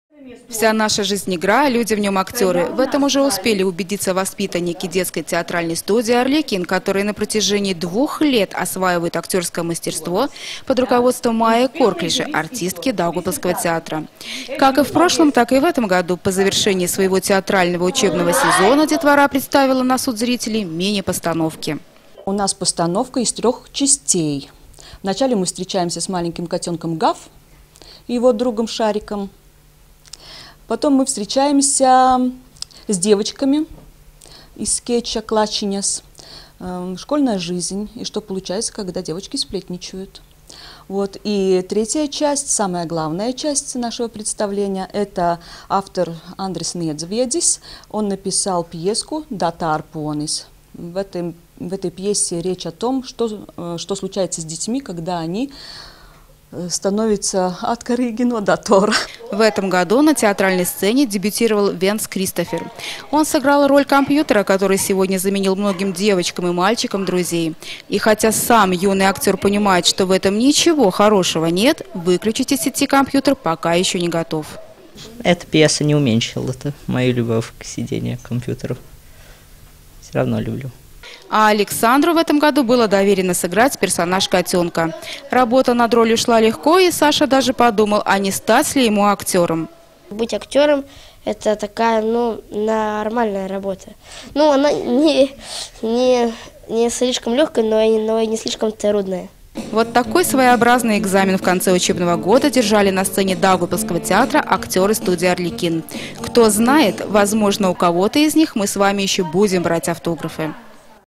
Студия «Арлекин» показала новые спектакли
22 мая сцена Даугавпилсского Государственного театра была отдана в полное распоряжение детворе. Театральная детская студия «Арлекин» представила на суд зрителей мини-спектакли, которые стали результатом еще одного года работы юных актеров. Подробности в нашем следующем сюжете.